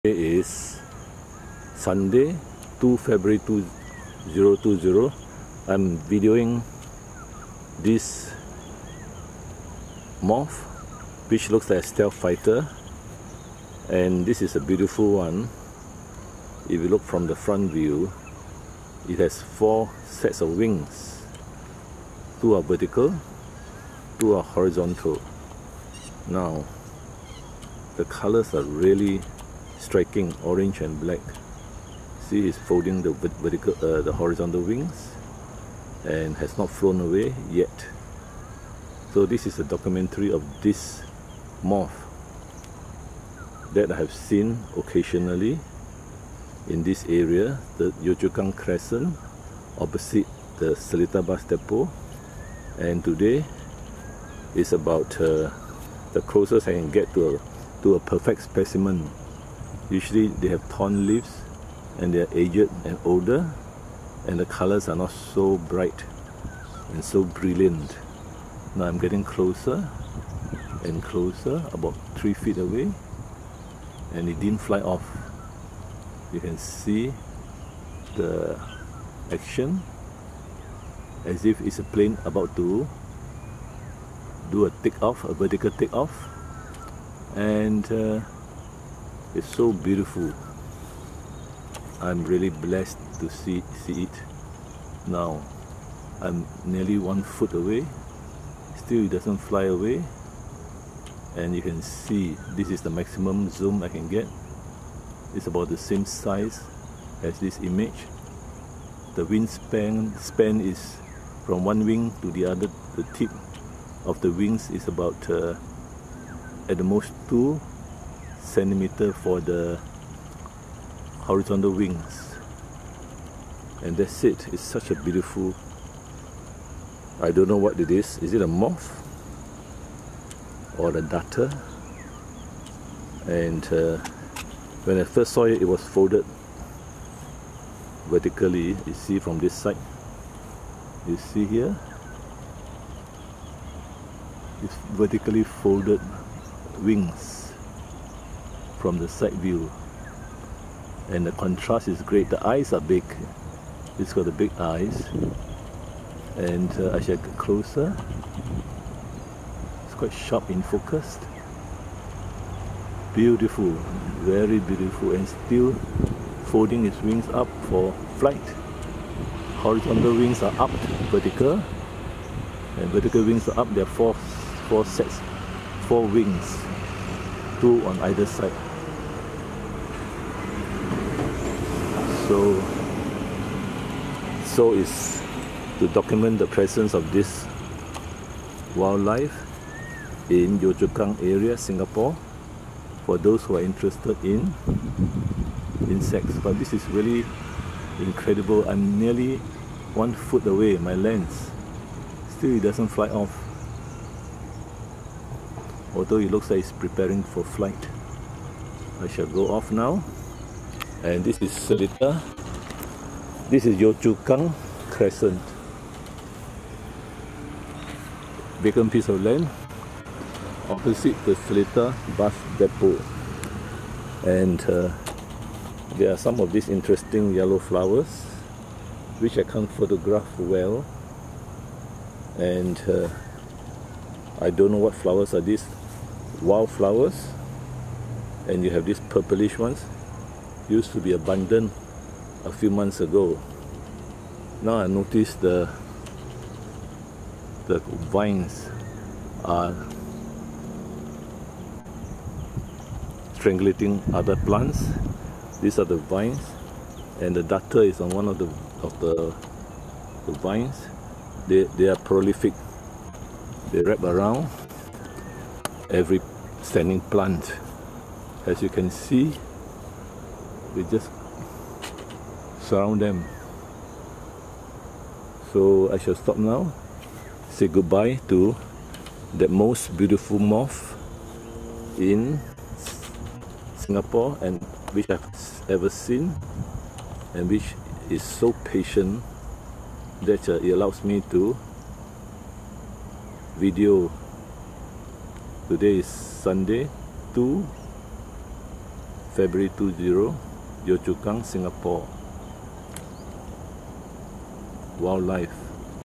Singapore Wildlife: An attractive orange-black moth with horizontal and vertical wings
Sunday 2 Feb 2020, I saw this attractive orange-black moth with vertical and horizontal wings. I saw two of such moths, both being young and in different locations.